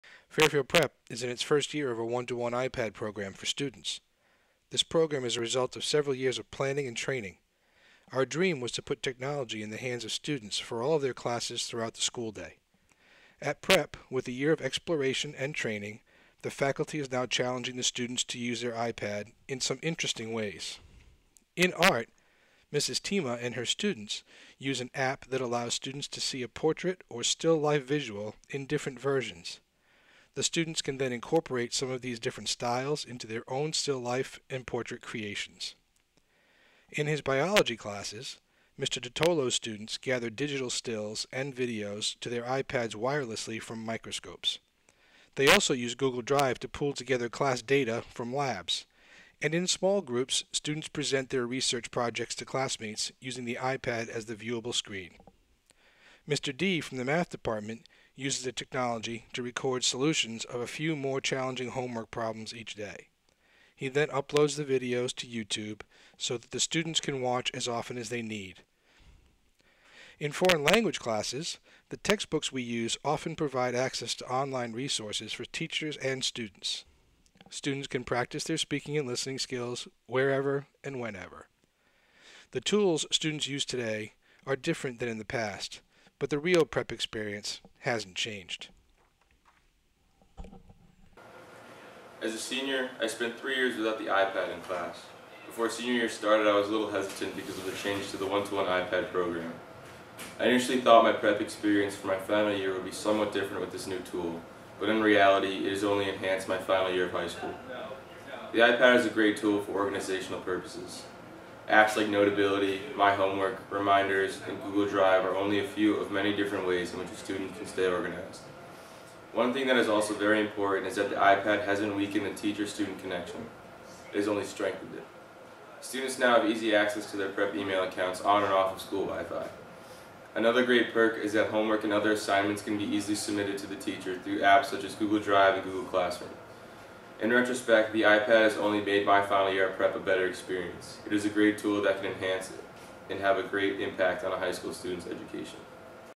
Fairfield Prep and the iPad1to1Program
Fairfield Prep integrates iPad technology to enhance its curriculum and offer our students and teachers the most advanced learning tools available.